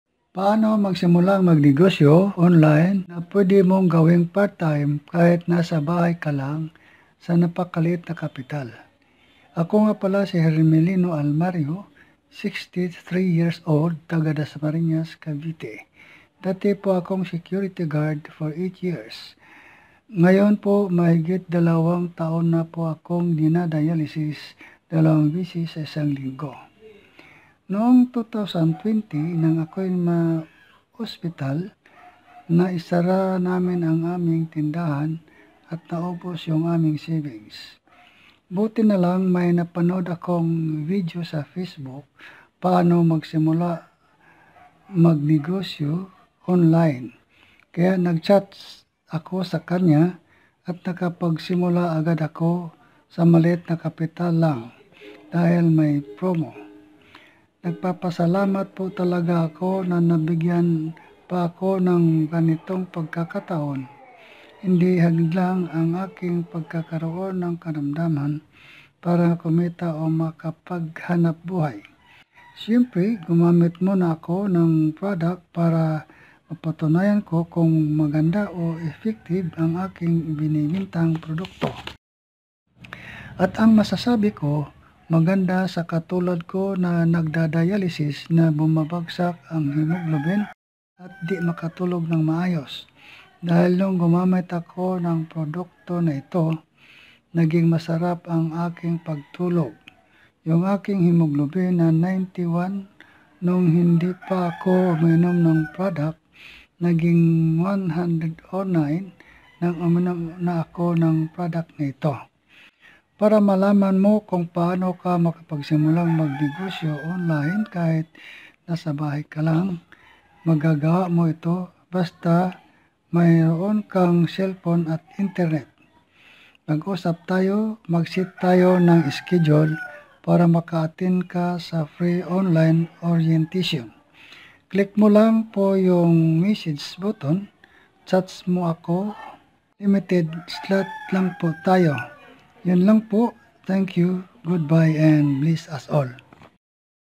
Paano mag negosyo online With Hermelino Almario v1
Gusto mo bang matuto kung Paano makapag simulang Magnegosyo Online sa napakaliit na Kapital lang?

Attend Our Free Online Orientation